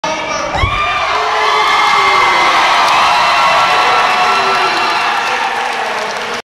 Foote Stomp!
Larry Foote at the Hines Ward show doing his "Foote Stomp"